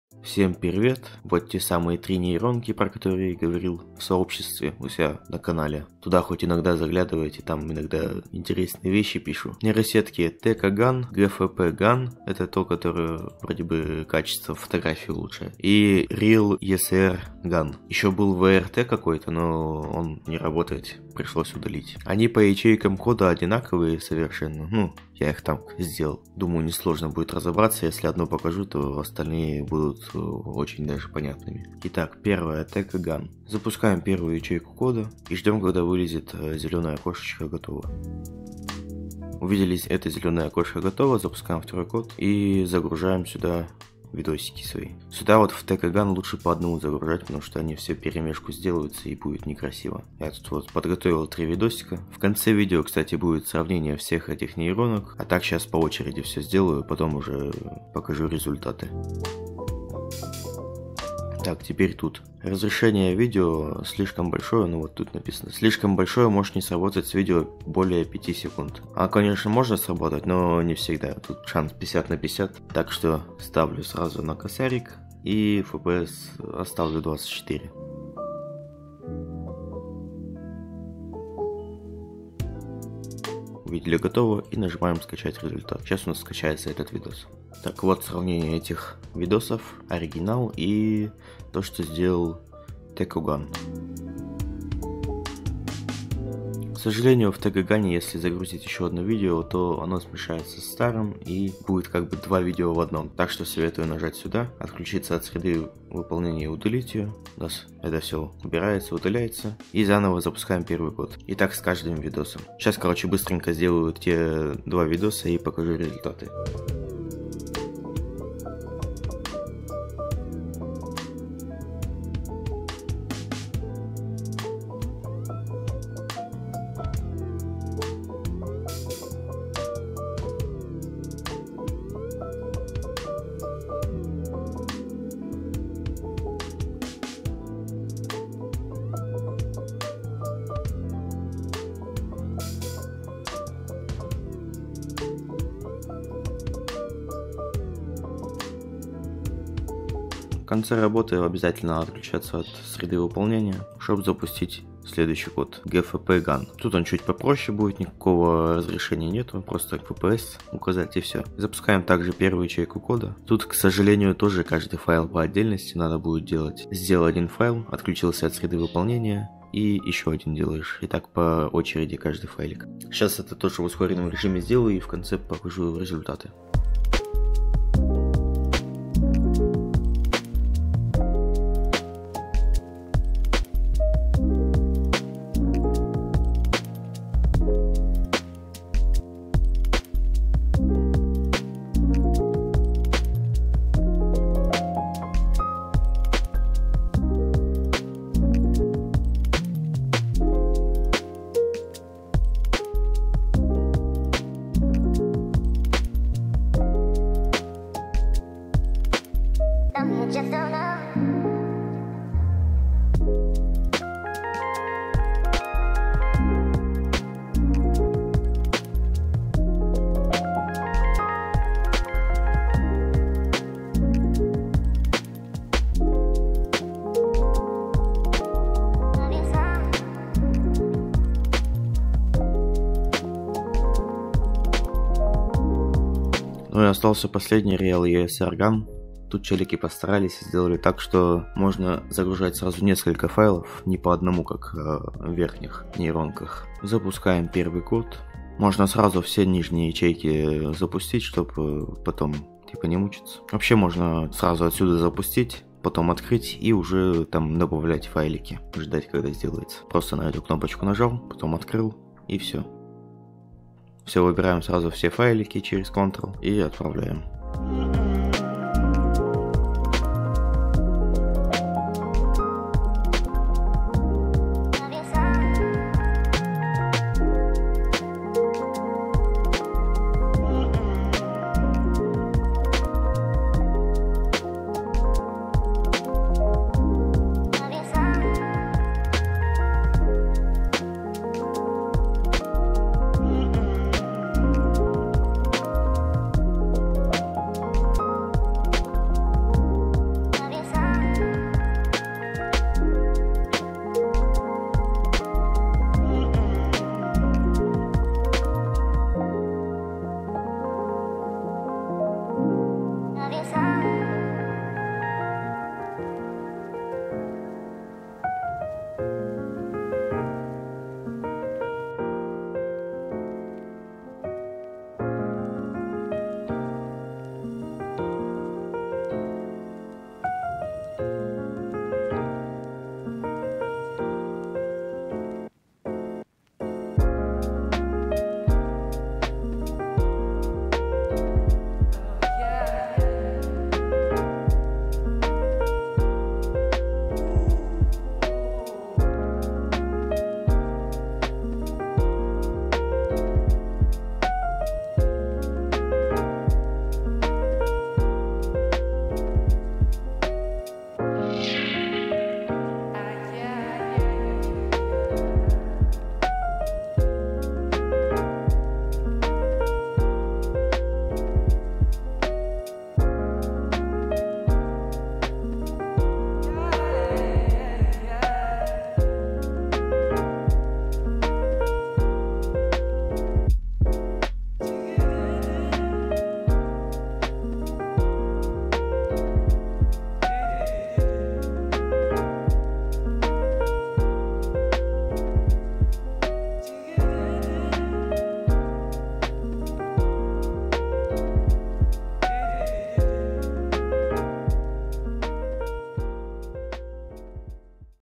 ПОВЫШАЕМ КАЧЕСТВО ВИДЕО в Colab | TecoGAN, GFPGAN и RealESRGAN
В этом видео я покажу, как улучшить качество изображения на видео с помощью трех различных нейронных сетей: TecoGAN, GFPGAN и RealESRGAN.

Много всяких дополнительных ссылок)

Обязательно подписываемся на всех, ну и конечно же на этот канал

0:00 - Приветствие
0:32 - TecoGAN
2:03 - Результат TecoGAN
2:35 - GFPGAN
3:19 - Результат GFPGAN
3:56 - RealERSGAN
5:08 - Результат RealERSGAN
5:48 - Сравнение всех результатов